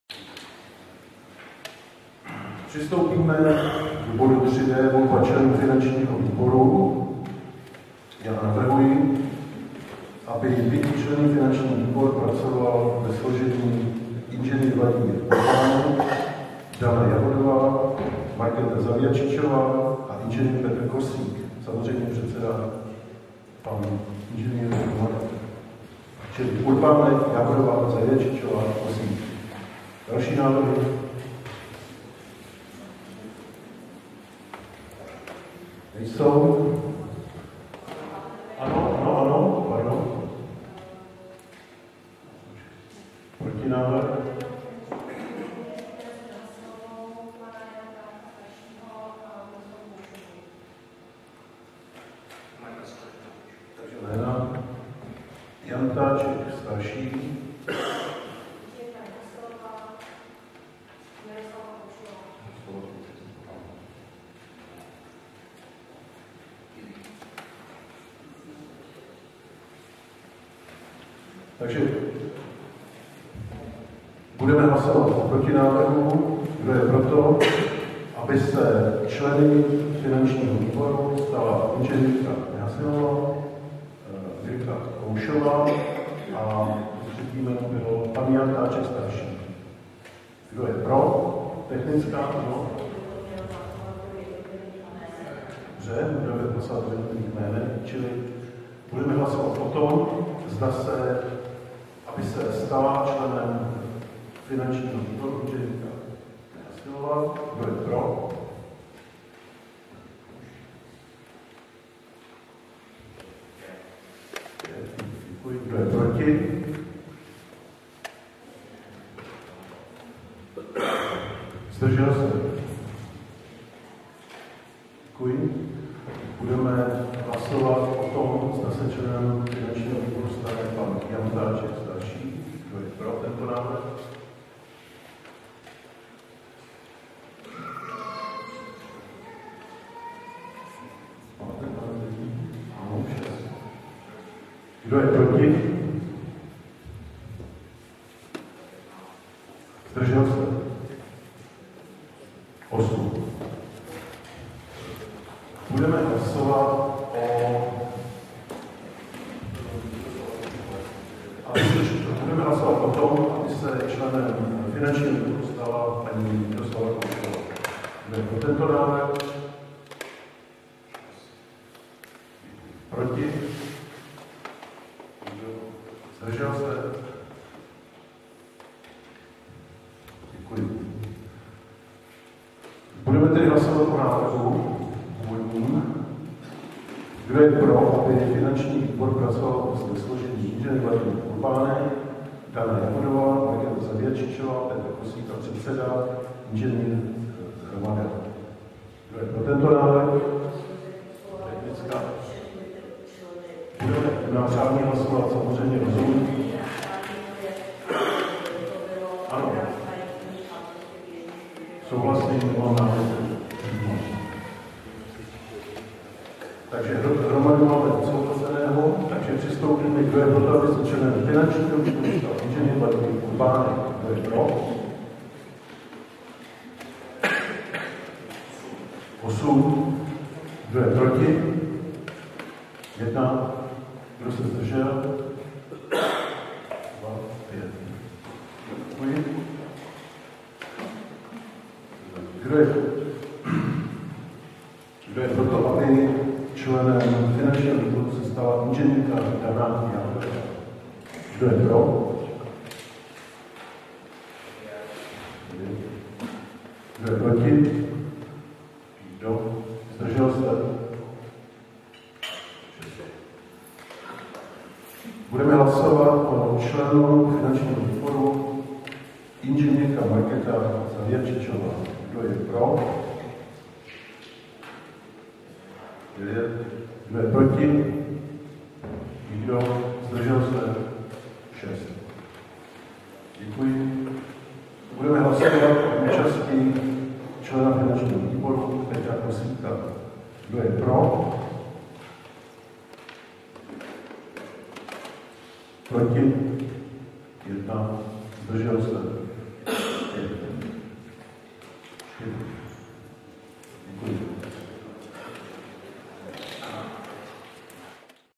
17 Volba členů finančního výboru
Záznam z jednání ustavujícího ZM Libčice nad Vlt, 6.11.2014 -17 Volba členů finančního výboru